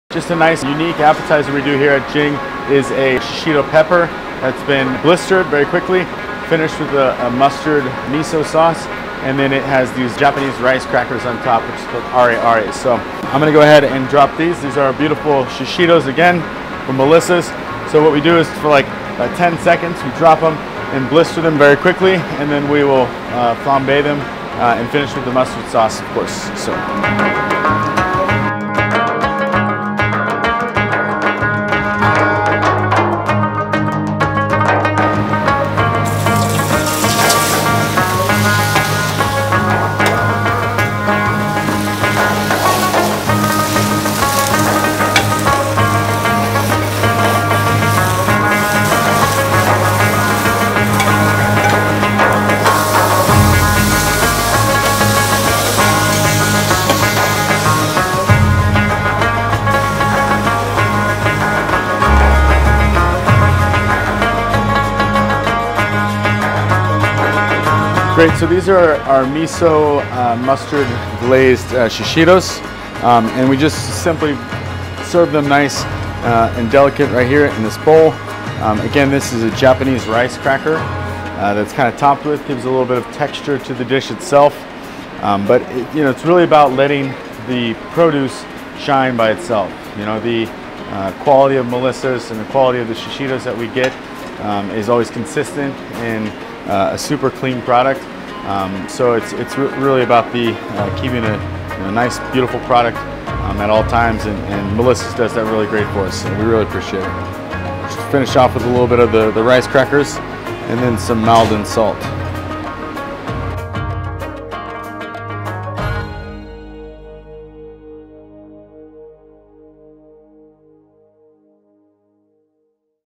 Jing Las Vegas | Shishito Pepper Appetizer
The Best Appetizer Ever @ Jing Las Vegas

Stopped by Jing Las Vegas, in Downtown Summerlin, to see how executive chef Thomas Griese likes to prepare Melissa’s Shishito Peppers. 🔥

If you’re local to Las Vegas, make sure to visit Jing or pick up shishito peppers at your local @albertsons  🛒